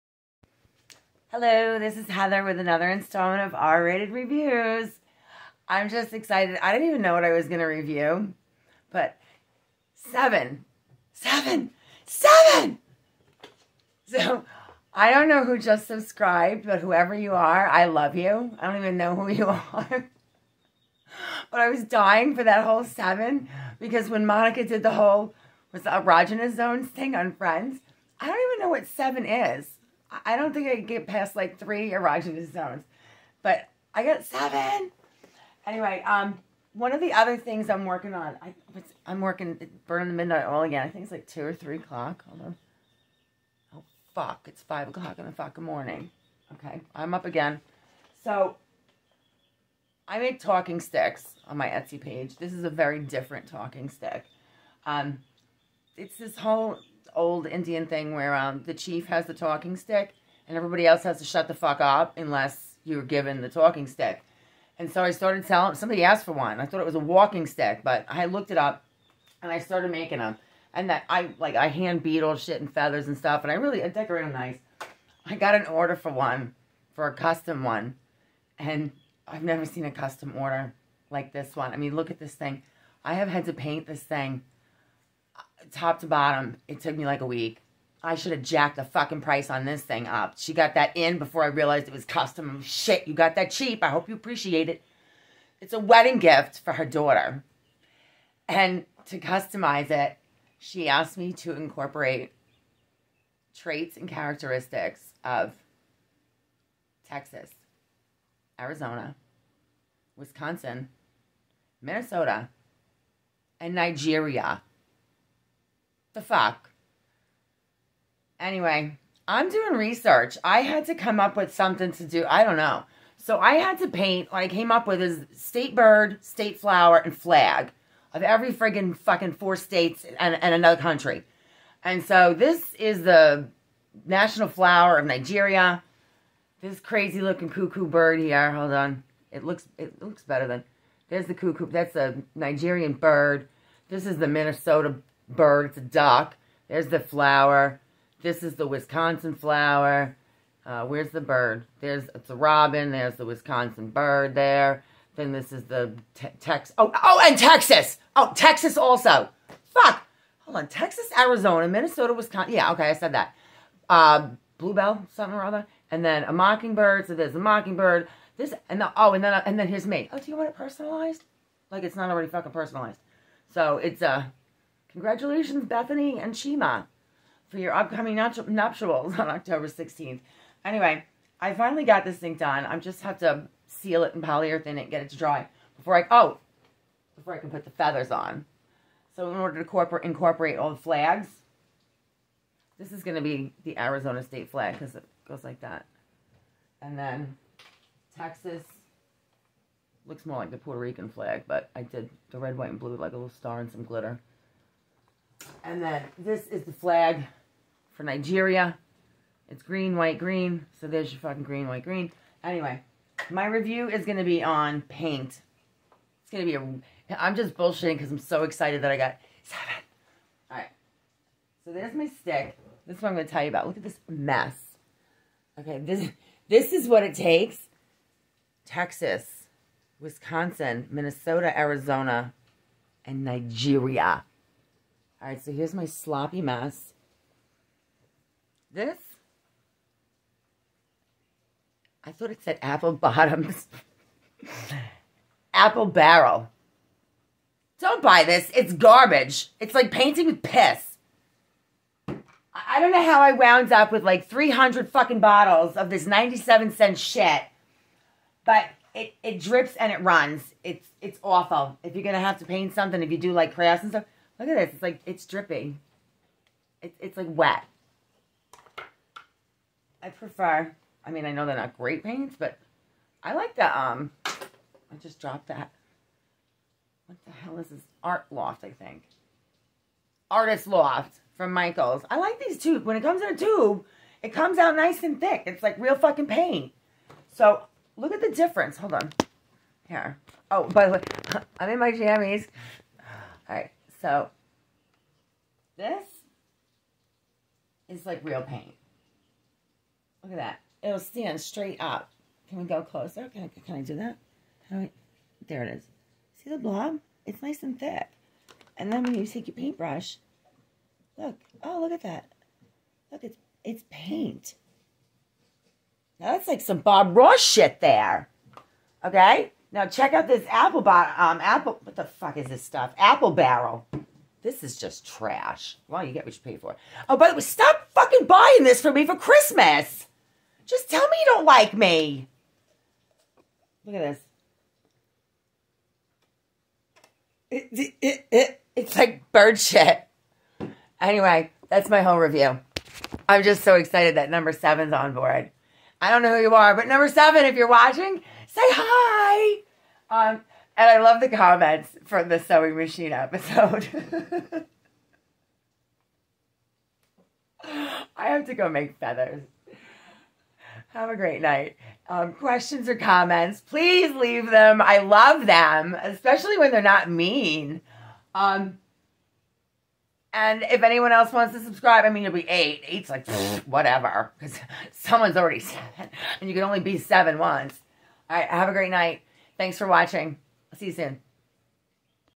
BS review for Apple Barrel Paint so I can say, Welcome 7! Keep an eye out for my coffee mug SEVEN!
I make custom talking sticks on my Etsy Page, heathershandmades916, along with a plethora of other unique items. As far as the paint necessary to create things, the cheap, trash paint from Walmart makes the job difficult. I did get a chuckle when I noticed my custom coffee mug in the background amongst my mess. 😉

$RRatedReviews